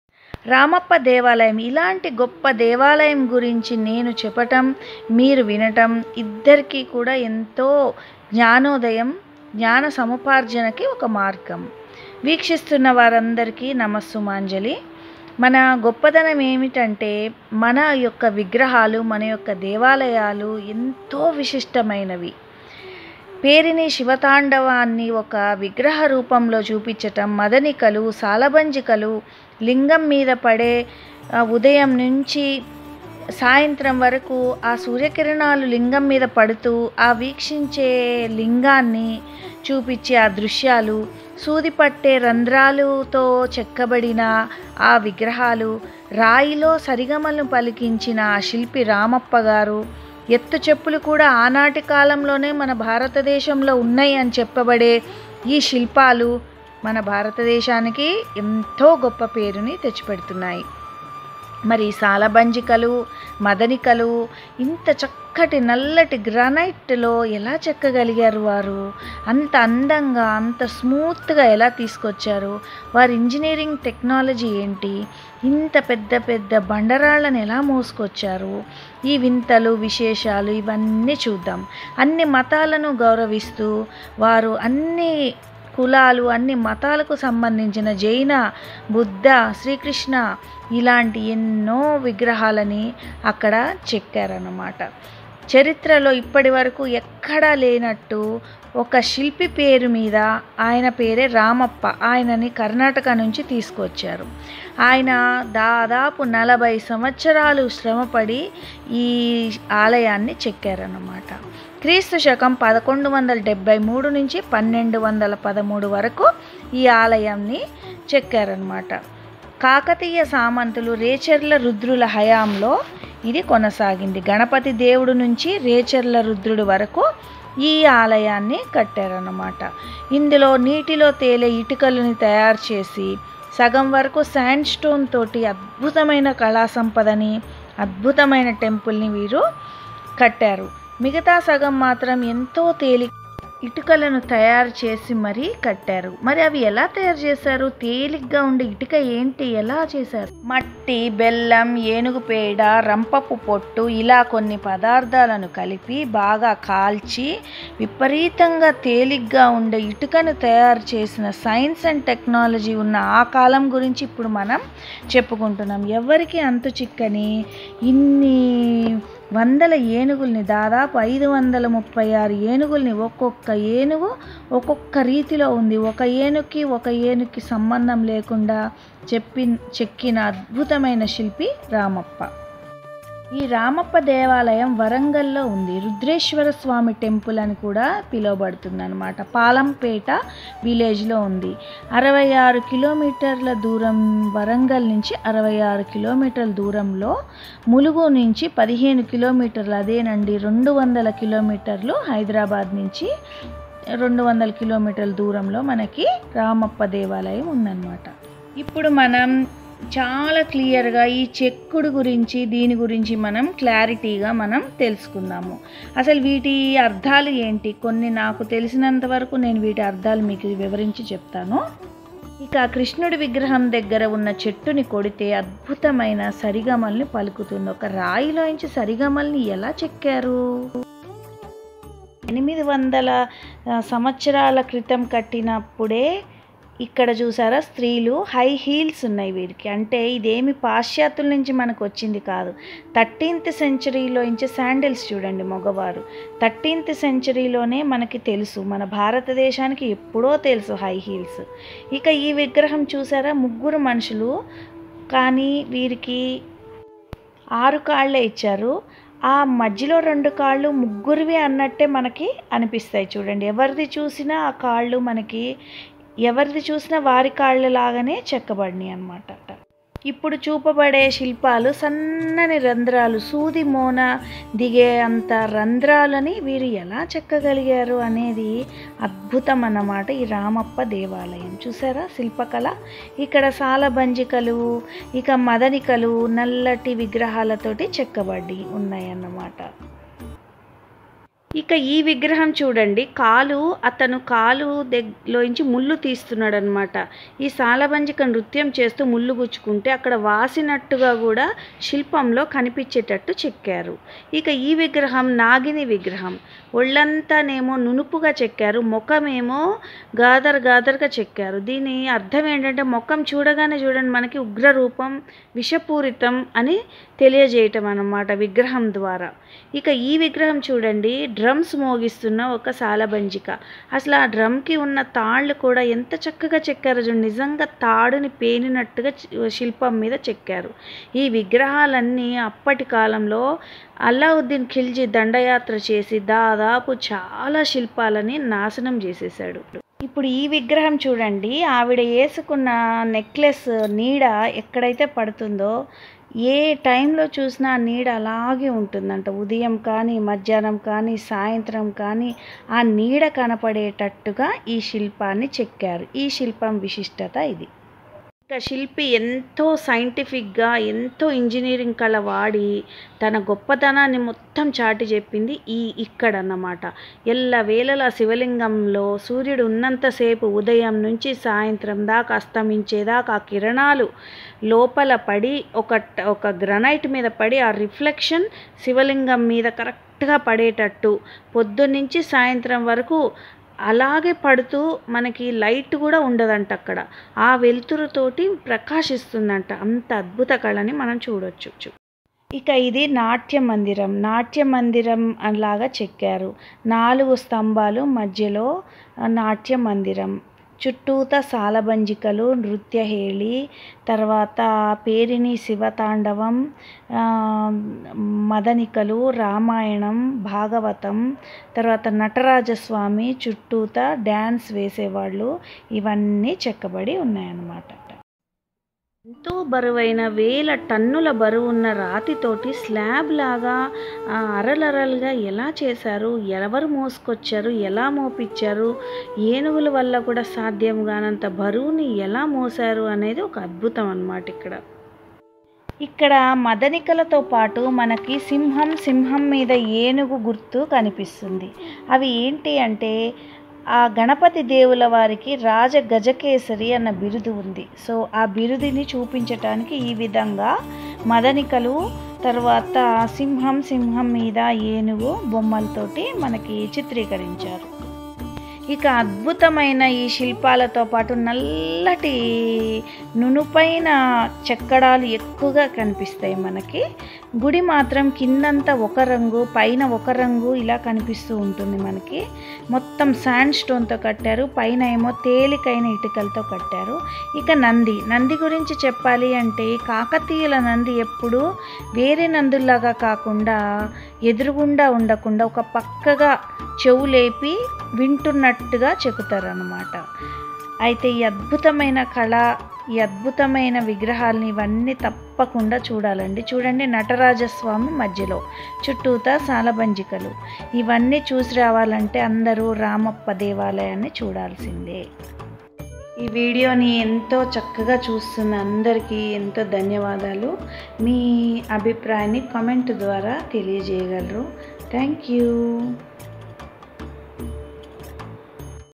The Secrets Of 800 Years Old Revealed || let's study the secrets of ramappa temple
The Secrets Of 800 Years Old Revealed ||ramappa temple ||Who Said High Heels are from west.. we know centuries ago.